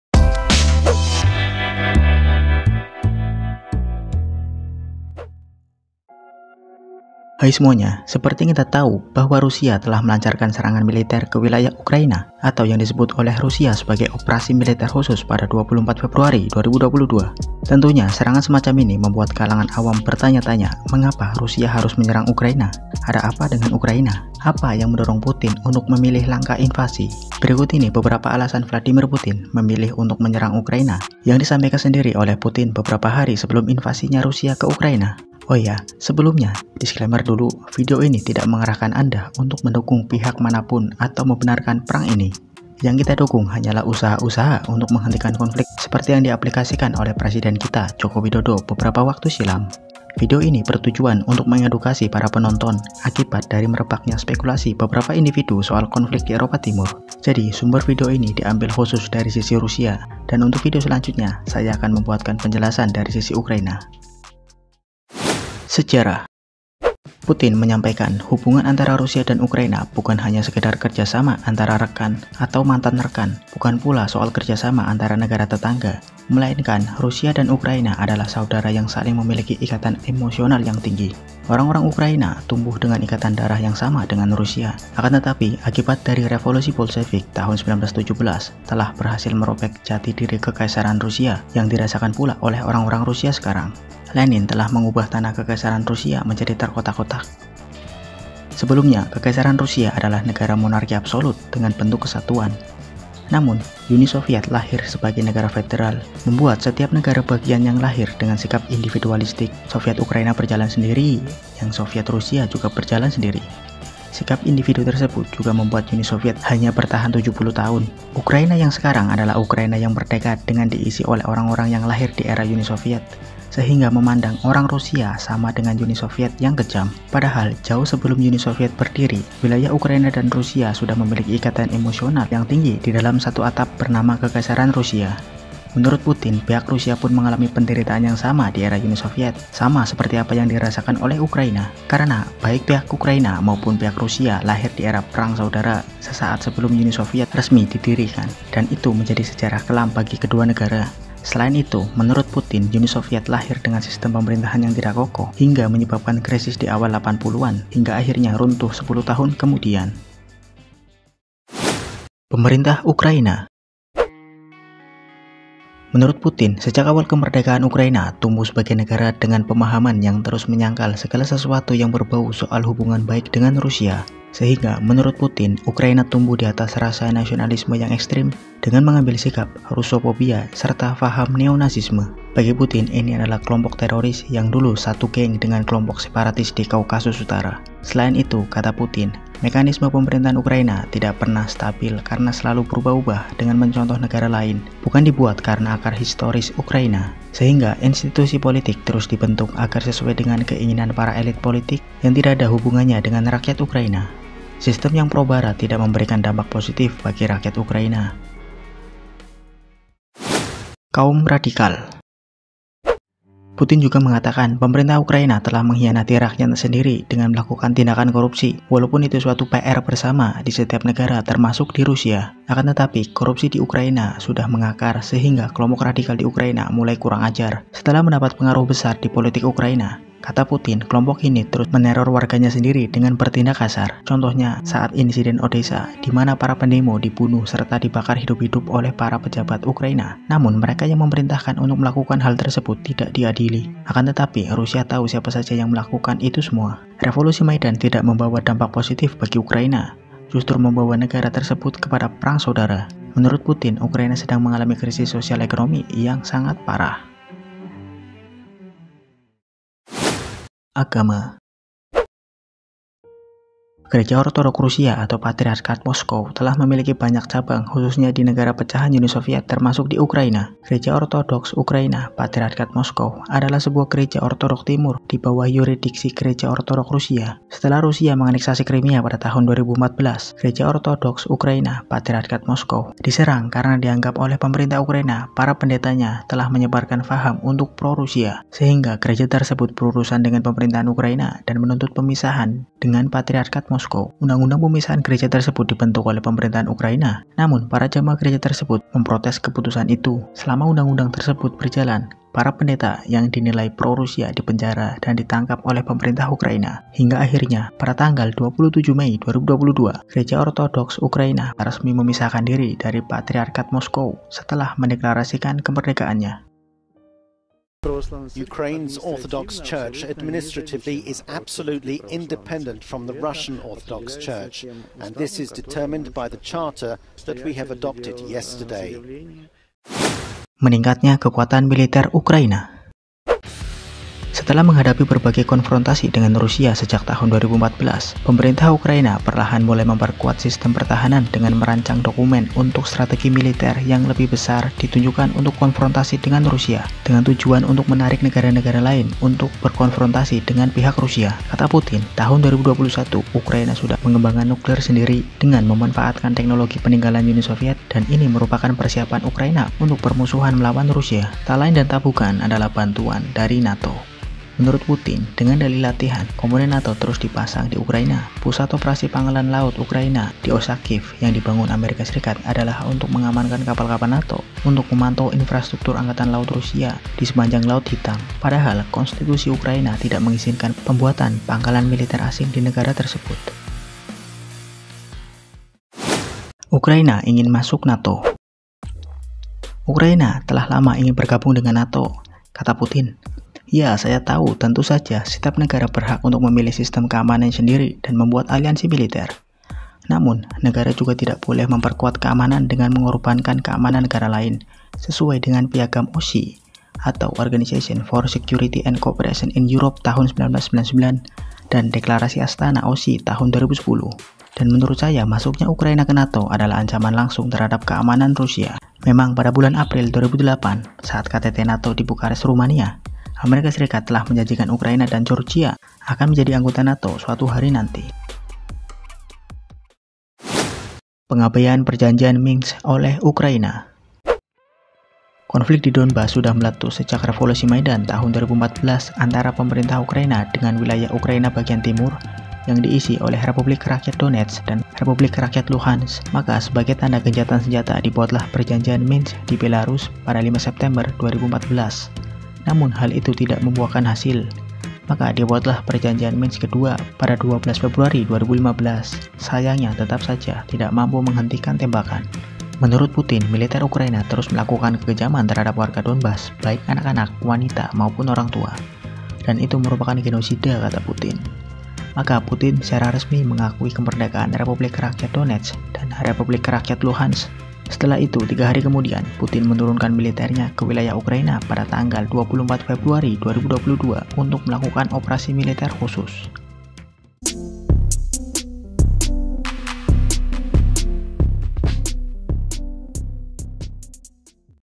Alasan Putin Menyerang Ukraina
Rusia sudah melancarkan invasinya ke Ukraina pasa 24 Februari 2022 yang disebut oleh Rusia sebagai Operasi Militer Khusus. Namun, banyak masyarakat awam termasuk saya ingin mengetahui sebenarnya apa yang mendasari Rusia memilih untuk menyerang Ukraina, apa tidak bisa lewat jalur diplomatik? Akhirnya saya coba cari tahu melalui beberapa pidato Putin sebelum melancarkan invasi. Dan saya rangkum berikut ini 7 alasan Rusia menyerang Ukraina. Tentunya hal ini berdampak begitu besar bagi masyar dunia khususnya bagi negara yang sedang berkonflik. sampai video ini dibuat perang kedua negara masih berlangsung. Kita doakan saja agar perang cepat berakhir dan dunia kembali normal.